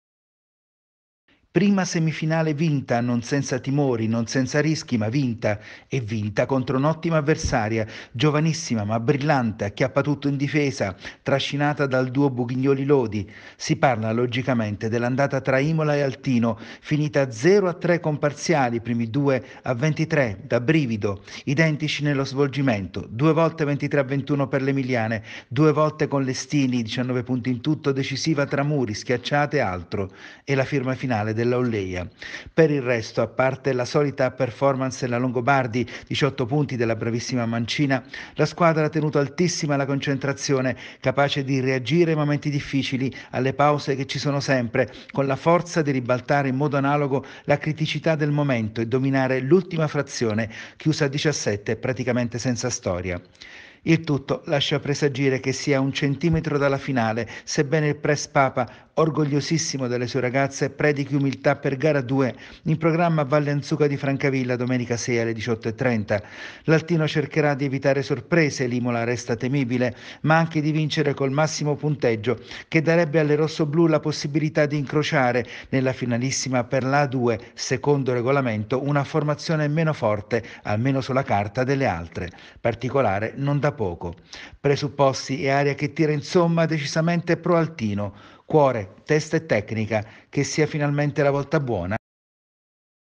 Volley. Altino a un passo dalla finale per la A2
E’ partita con il piede giusto l’Altino Volley che si è aggiudicata gara 1 di semifinale a Imola per 3 set a zero. Domenica alle 18:30 a Francavilla si disputerà gara 2. Per il sestetto abruzzese c’è la possibilità di chiudere subito i giochi e volare in finale